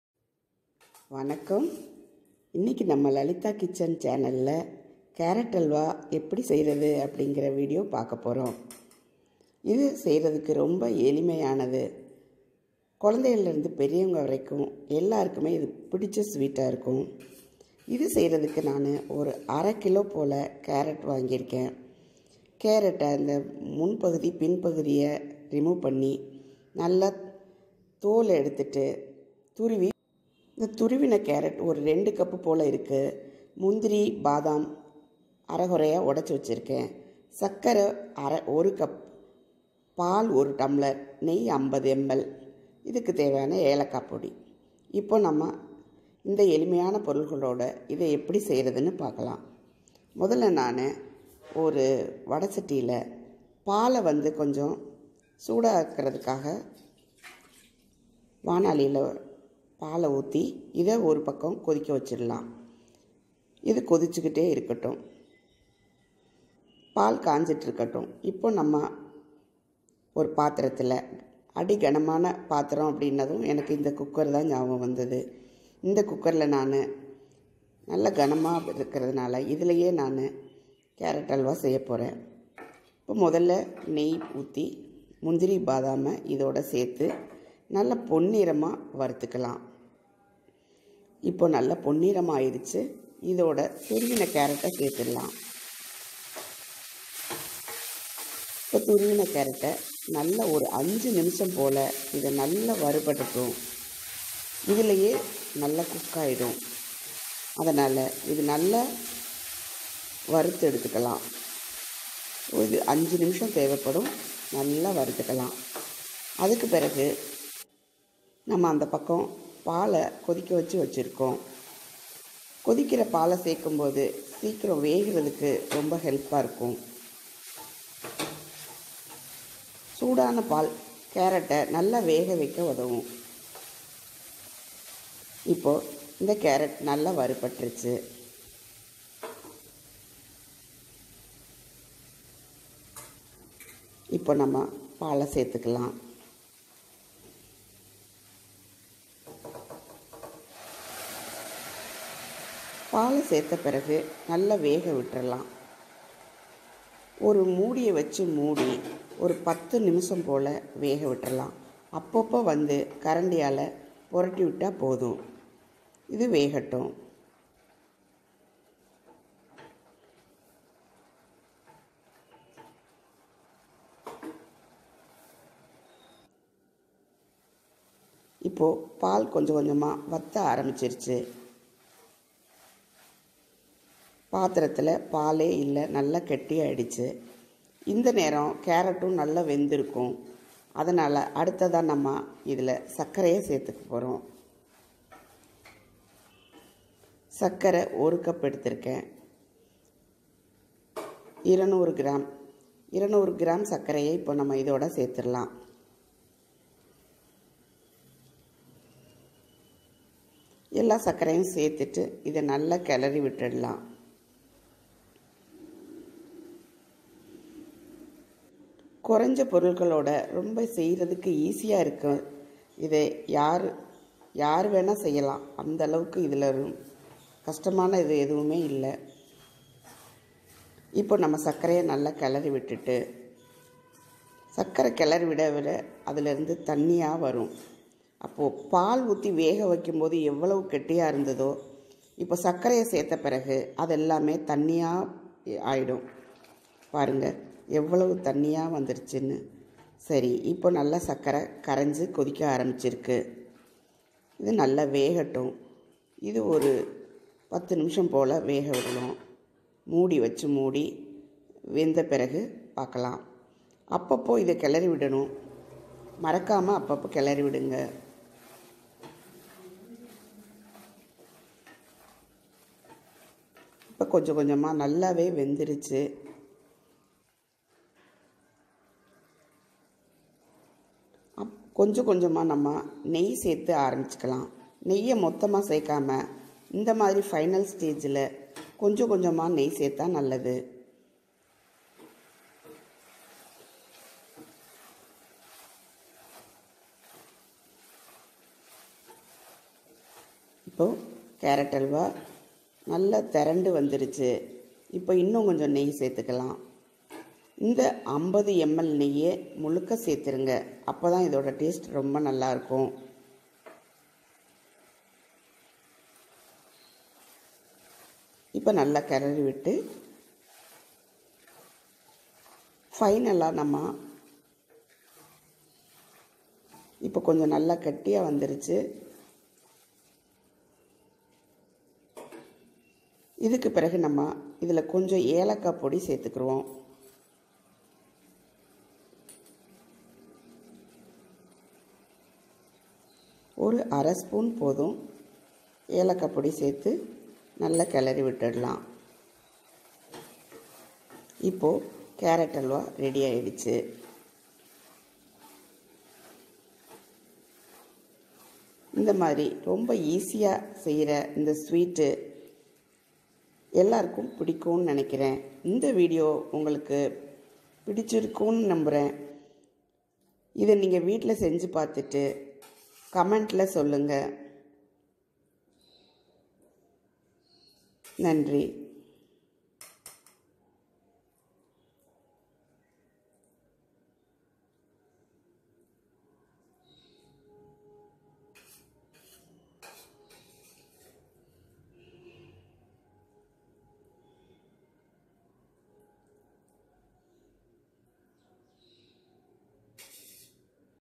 Carrot halwa at home கேரட் அல்வா lalitha kitchen
சுவையான கேரட் அல்வா, எளிய பொருட்களுடன் easy யா வீட்டிலேயே செய்யலாம்
Lalitha kitchen

lalitha kitchen